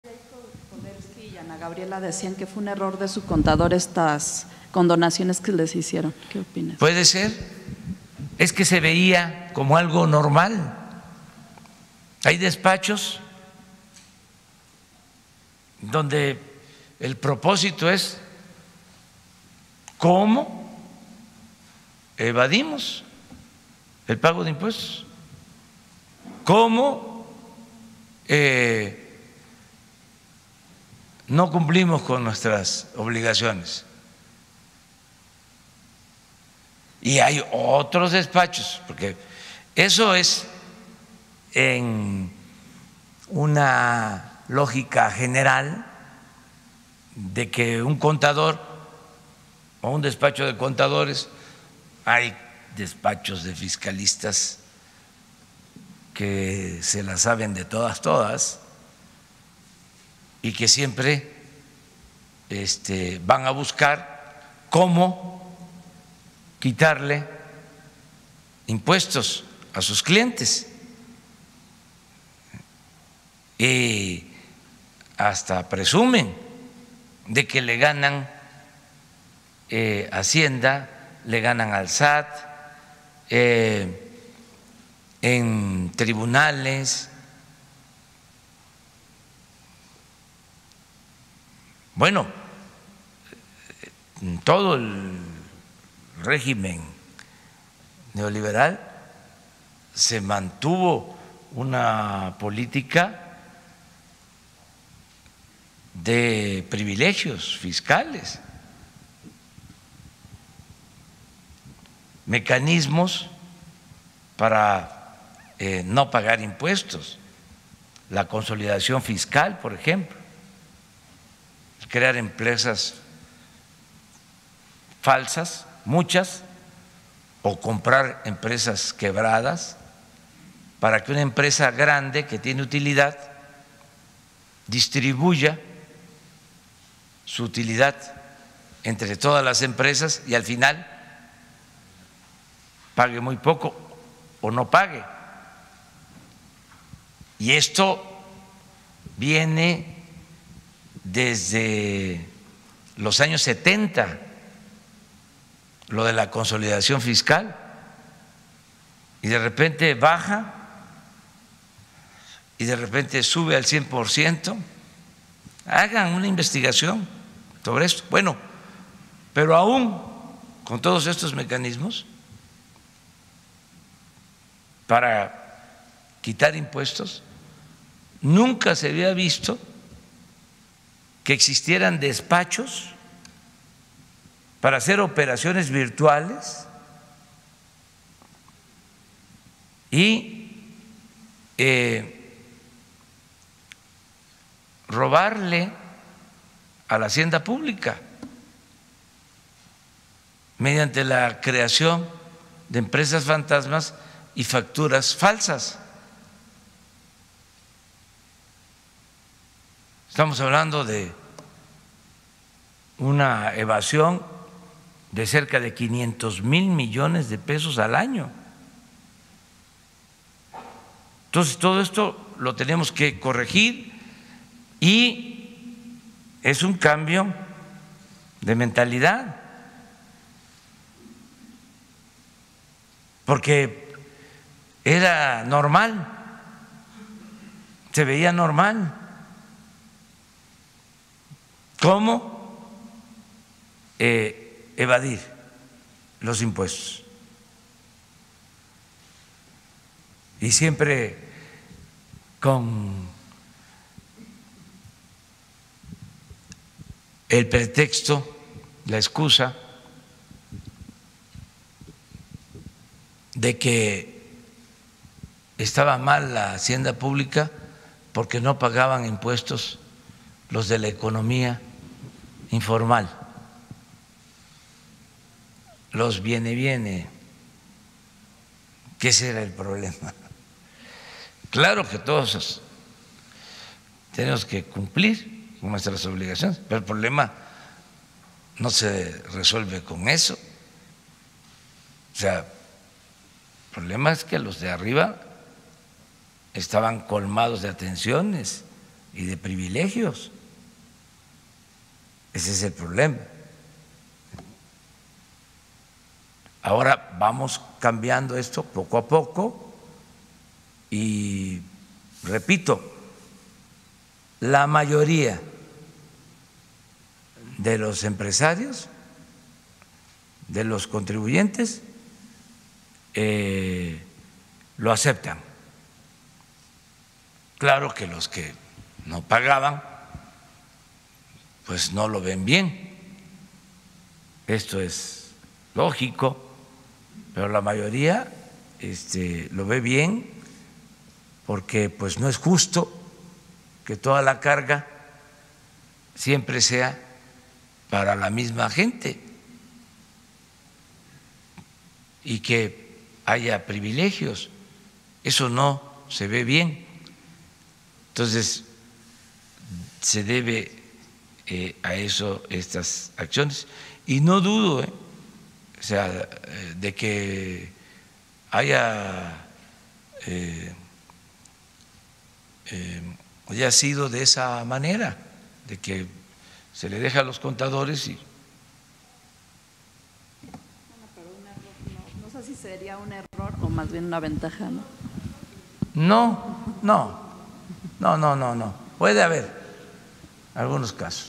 AMLO COMENTA QUE EN CASO DE YEIDCKOL Y GUEVARA, ERROR “PUEDE SER”
López Obrador le respondió a la reportera de Ciro Gómez Leyva para Radio Fórmula que “puede ser” que en el caso de Yeidckol Polevnsky y Ana Gabriela Guevara haya sido un error eso de que les condonaran millones de pesos en impuestos en el gobierno de Peña Nieto… Que hay despachos en los que el propósito es evadir impuestos, que hasta presumen que le ganan a Hacienda en tribunales